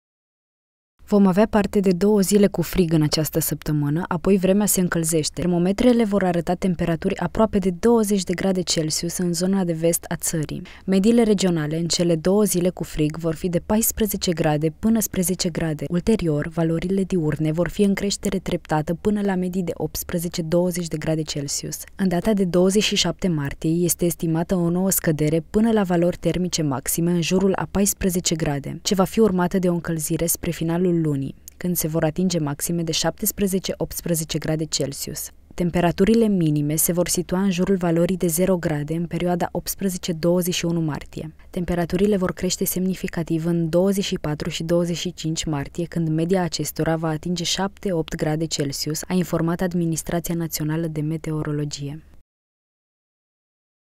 Două zile friguroase în această săptămână
Vom avea parte de două zile cu frig în această săptămână, apoi vremea se încălzește. Termometrele vor arăta temperaturi aproape de 20 de grade Celsius, în zona de vest a țării. Mediile regionale în cele două zile cu frig vor fi de 14 grade, până spre 10 grade. Ulterior, valorile diurne vor fi în creștere treptată până la medii de 18-20 grade celsius. În data de 27 martie este estimată o nouă scădere până la valori termice maxime în jurul a 14 grde, ce va fi urmată de o încălzire spre finalul lunii, când se vor atinge maxime de 17-18 grade celsius